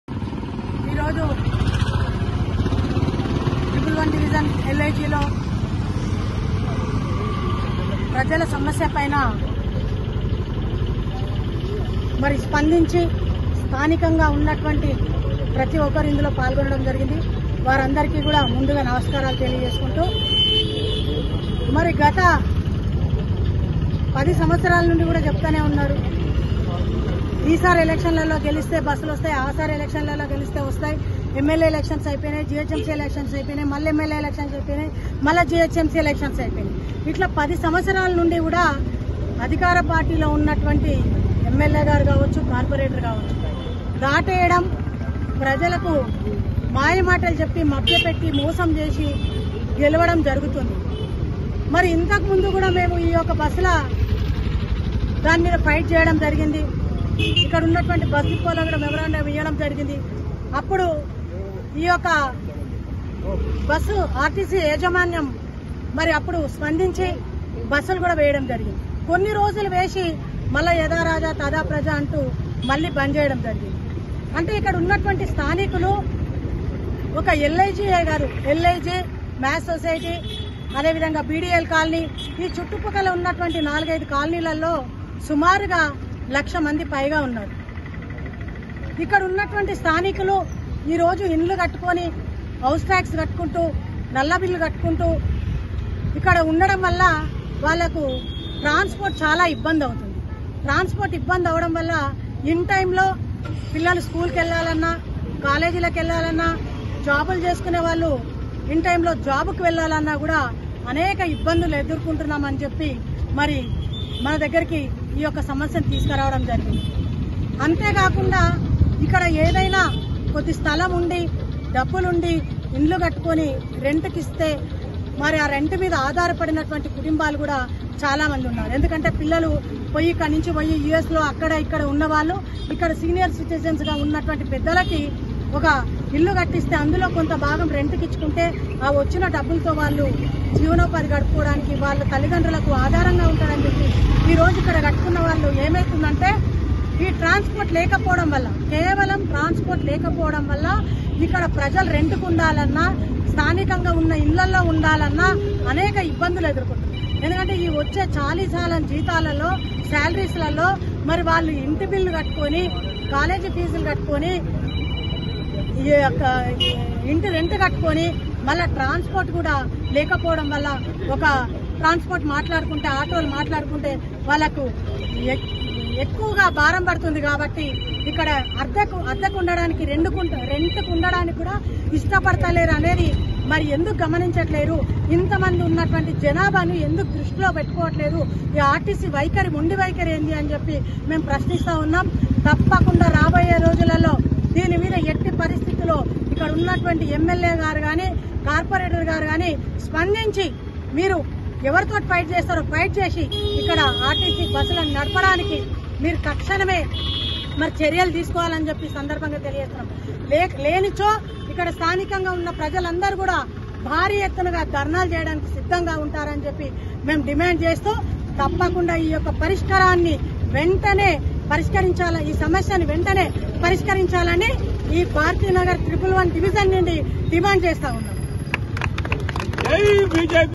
మా LIG కి RTC బస్సు కావాలీ అని BJP నాయకు రాలు గోదావరి అంజిరెడ్డి..
Video from Lc